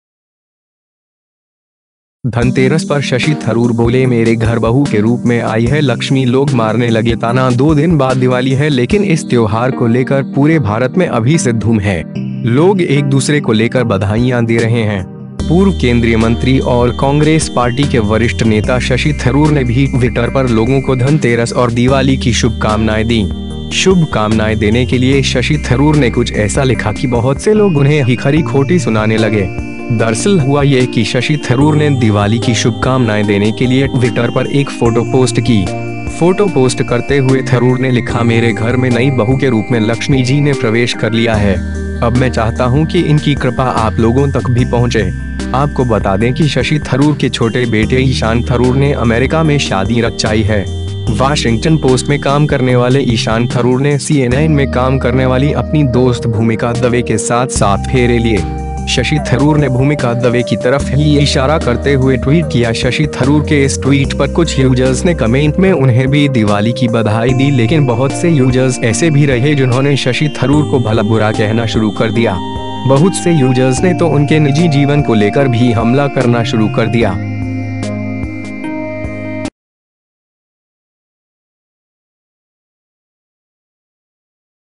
धनतेरस पर शशि थरूर बोले मेरे घर बहू के रूप में आई है लक्ष्मी, लोग मारने लगे ताना
दो दिन बाद दिवाली है लेकिन इस त्योहार को लेकर पूरे भारत में अभी से धूम है। लोग एक दूसरे को लेकर बधाईयां दे रहे हैं। पूर्व केंद्रीय मंत्री और कांग्रेस पार्टी के वरिष्ठ नेता शशि थरूर ने भी ट्विटर पर लोगों को धनतेरस और दीवाली की शुभकामनाएं दीं। शुभकामनाएं देने के लिए शशि थरूर ने कुछ ऐसा लिखा कि बहुत से लोग उन्हें ही खरी-खोटी सुनाने लगे। दरअसल हुआ ये कि शशि थरूर ने दिवाली की शुभकामनाएं देने के लिए ट्विटर पर एक फोटो पोस्ट की। फोटो पोस्ट करते हुए थरूर ने लिखा- मेरे घर में नई बहू के रूप में लक्ष्मी जी ने प्रवेश कर लिया है, अब मैं चाहता हूं कि इनकी कृपा आप लोगों तक भी पहुंचे। आपको बता दें कि शशि थरूर के छोटे बेटे ईशान थरूर ने अमेरिका में शादी रचाई है। वाशिंगटन पोस्ट में काम करने वाले ईशान थरूर ने सीएनएन में काम करने वाली अपनी दोस्त भूमिका दवे के साथ सात फेरे लिये। शशि थरूर ने भूमिका दवे की तरफ ही ईशारा करते हुए ट्वीट किया।शशि थरूर के इस ट्वीट पर कुछ यूजर्स ने कमेंट में उन्हें भी दिवाली की बधाई दी लेकिन बहुत से यूजर्स ऐसे भी रहे जिन्होंने शशि थरूर को भला बुरा कहना शुरू कर दिया। बहुत से यूजर्स ने तो उनके निजी जीवन को लेकर भी हमला करना शुरू कर दिया।